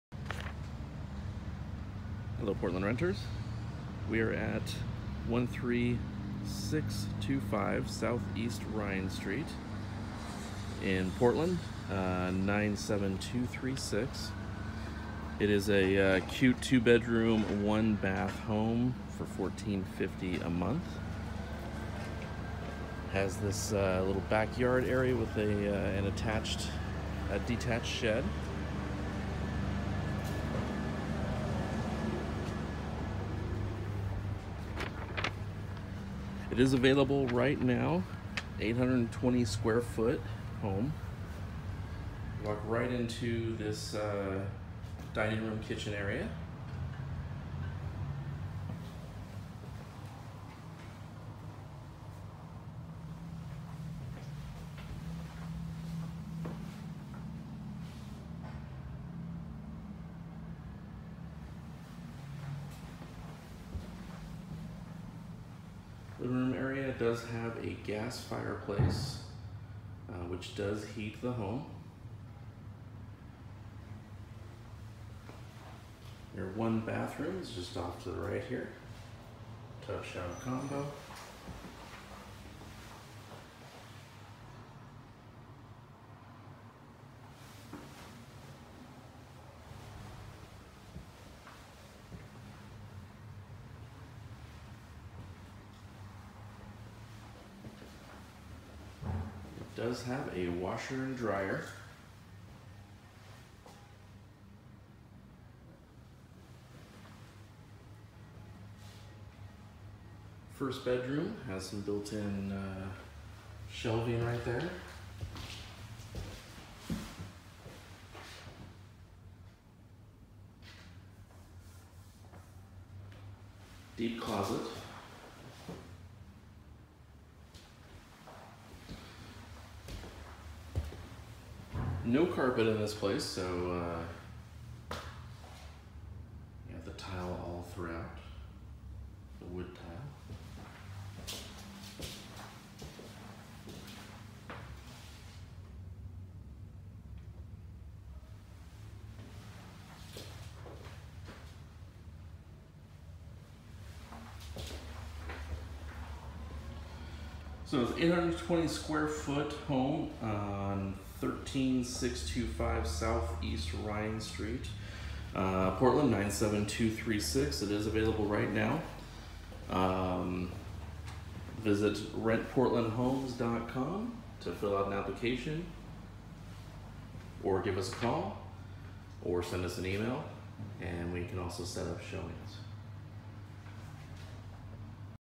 For Rent: 13625 SE Rhine St, Portland
This little home is light and bright. 2 bedroom, 1 full bathroom. Kitchen, dining and extra large living room. Yard is well maintained. Covered front porch. Shed for extra storage. Large laundry room with washer and dryer included.

RentPortlandHomes.com

Do you need property management services?
Maximize your income and cut your costs!